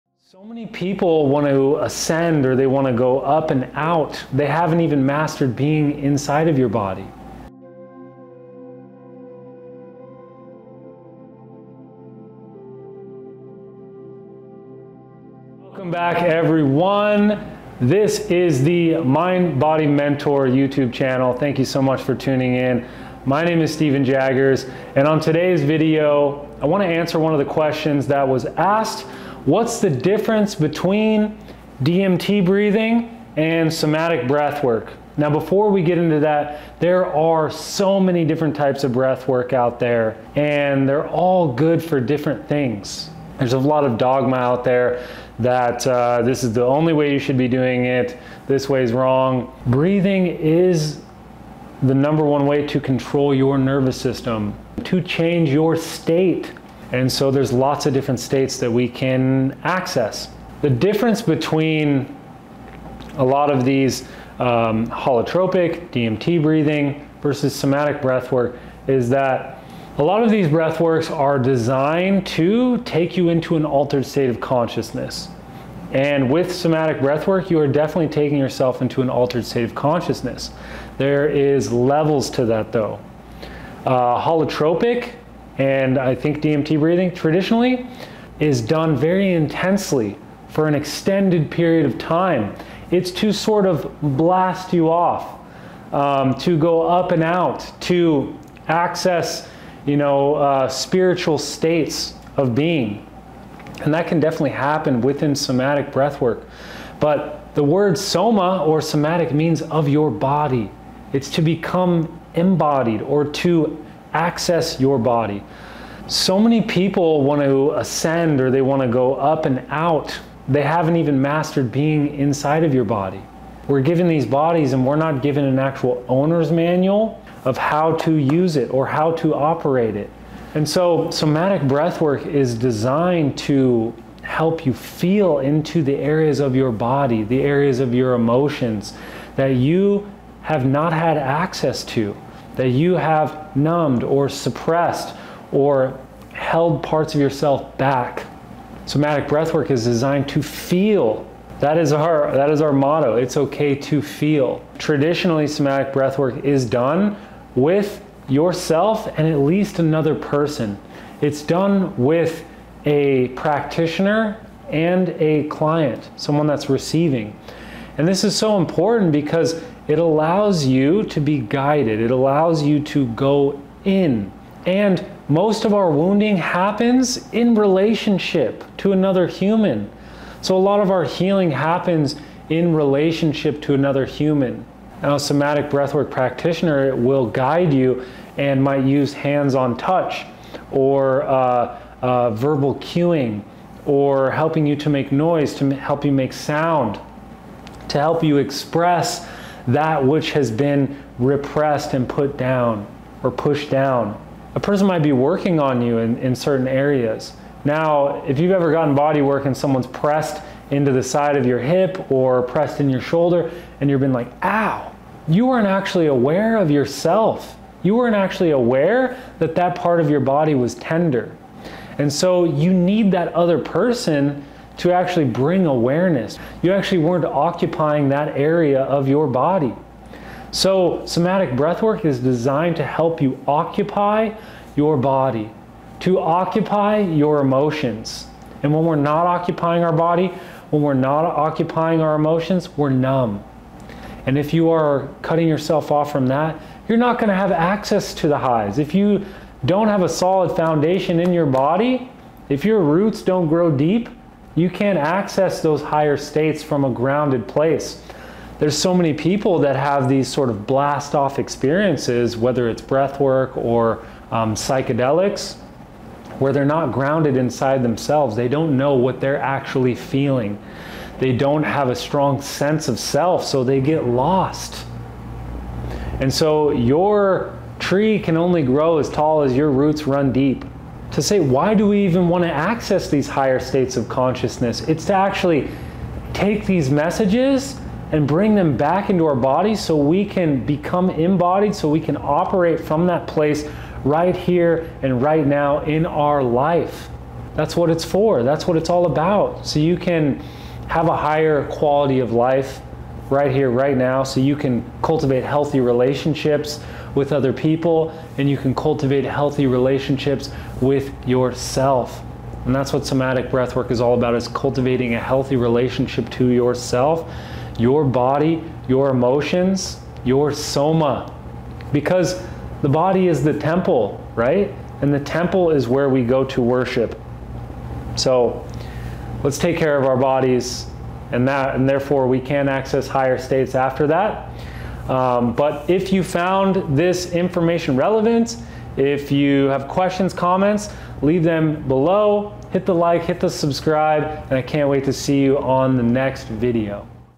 DMT Breathing Vs Experiencing Somatic Breathwork
📅UPCOMING Breathwork Trainings

Many individuals familiar with psychedelic experiences often find parallels within specific breathwork techniques that are collectively referred to as DMT breathing or holotropic breathwork.

Tapping into the DMT naturally produced in our bodies, breathwork modalities like holotropic leverage stressful bodily states as a means to release DMT naturally in the participant, though such a delivery mechanism has yet to be scientifically proven in human beings.

Breathwork sessions that are commonly associated with the release of DMT in the body typically leverage rapid changes between c02 and oxygen levels in the body as a means to create the stress needed to release DMT via the induction of stress created during breathing exercises.

Because rapid breathing can cause blood vessels to narrow, a decreased oxygen level during holotropic breathwork may create the DMT release via mild hypoxia. Derived from scientific studies showing that DMT has the ability to protect cells from dying in reduced oxygen environments, one could infer that inducing such a situation in our body can lead to the natural release of DMT via breathwork modalities like holotropic.

As students seeking to experience DMT breathing are often seeking an elevated, heightened state of consciousness via breathwork, many have failed to create a solid foundation to facilitate such a state and are often left with a confusing session. Despite seeking to expand consciousness or have a psychedelic experience, many people who have unresolved energies in their nervous system fail to achieve to achieve such an expansive experience.

While many will still sense an altered state of consciousness, experiencing Somatic breathwork differs from DMT breathing or holotropic because students are seeking to go within themselves to resolve trapped traumatic or energies that are causing dysfunction in their daily lives. By targeting our Soma breathing exercises are both intense and subdued, as a means to exhale trapped energies from our nervous system while also inhaling positive energies into our bodies.

While experiencing Somatic breathwork may be less of a psychedelic feeling for most, that vast majority of students who participate in guided breathwork sessions targeting their Soma recount life changing effects that often lead to a better existence and the eradication of many trauma responses that may unknowingly be plaguing them.

00:00 DMT Breathing Vs Somatic Breathwork
00:36 Uses for Varied Breathwork Modalities
01:00 How Somatic Breathwork Differs
03:05 Nature of Guided Breathwork Experiences
03:58 Accessing Higher States of Consciousness